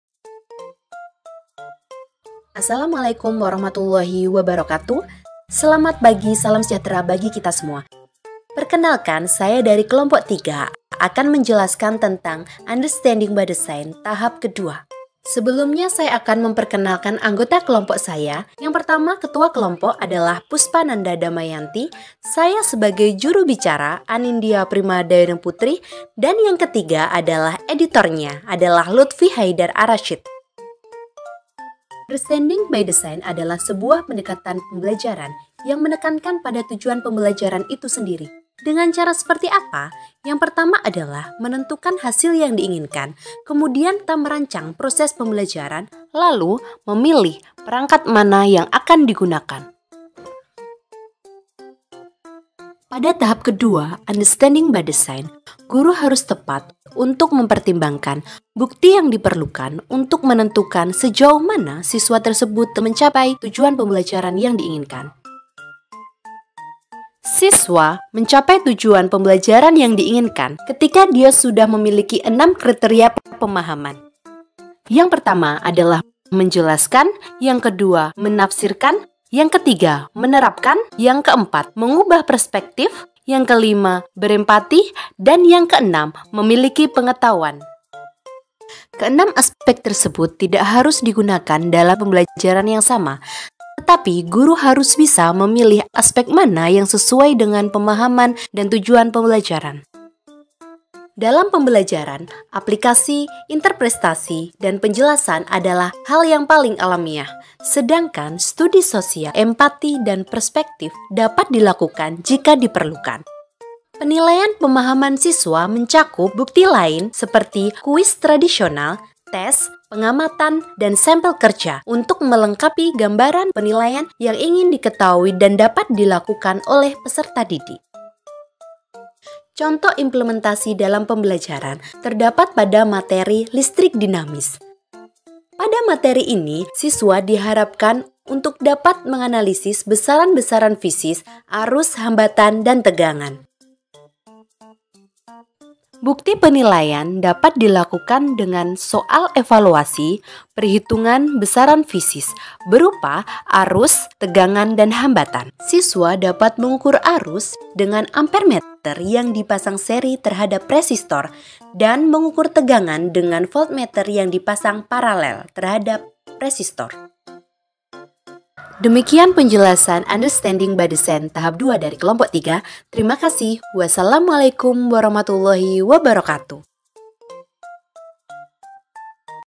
Kelompok 3 UbD (Understanding by Design): Tentukan Bukti Penilaian
Dipersentasikan oleh Kelompok 3 Mahasiswa PPG Prajabatan Bidang Studi IPA Universitas PGRI Madiun Tahun 2022:
1. Anindya Primadayuning Putri (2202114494)
2. Luthfy Haidar A (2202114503)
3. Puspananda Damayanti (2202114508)